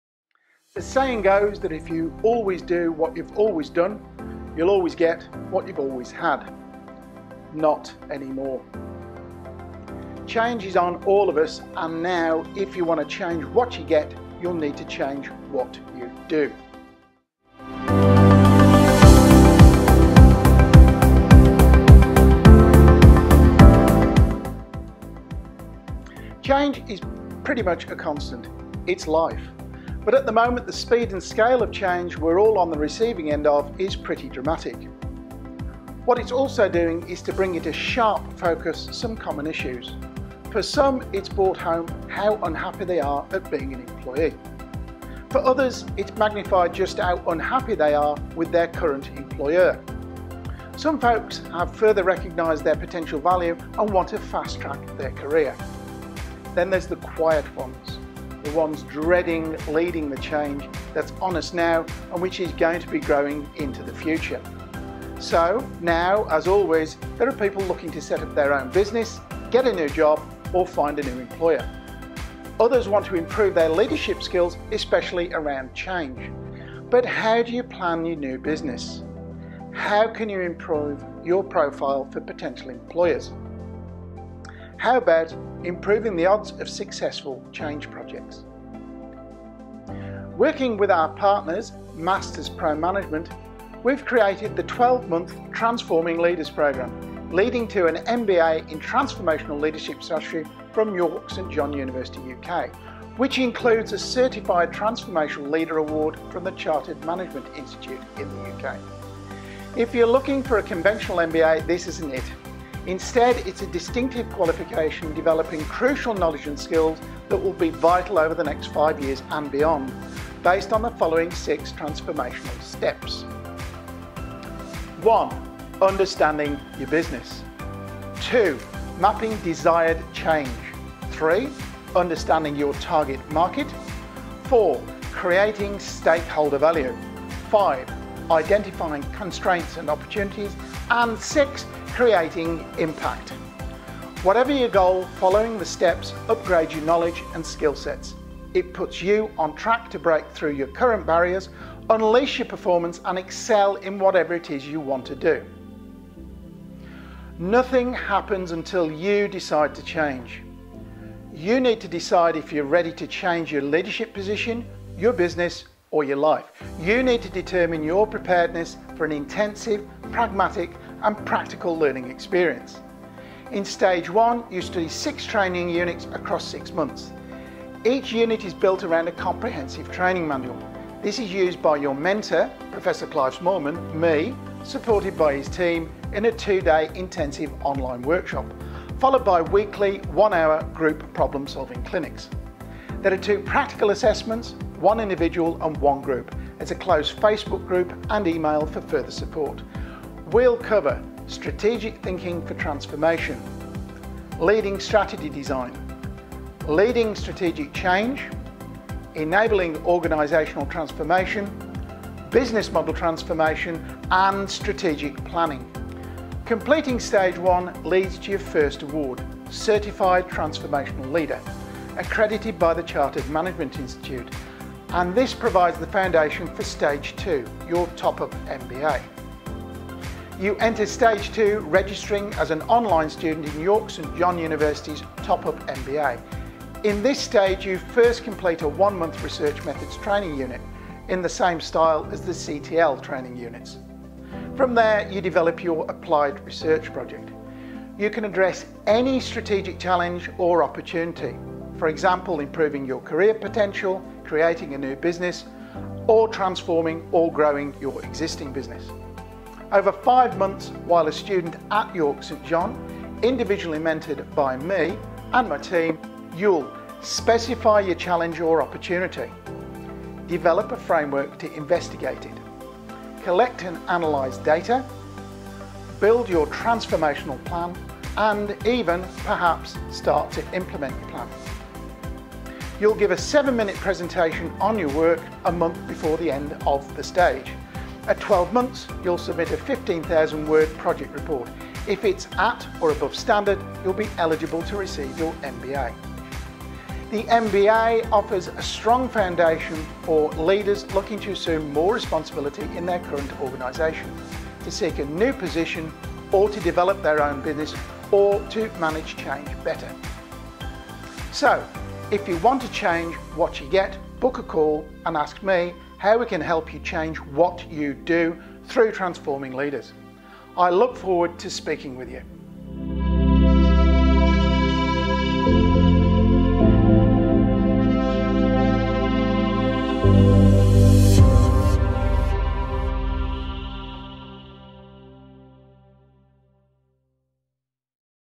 Transforming Leaders: The Programme
Prof Clive Smallman introduces the Transforming Leaders mentoring programme Focused on transformational leadership, this 12-month intensive, pragmatic and practical learning experience prepares you to find a new role, fast-track your career, develop your own business or manage change better.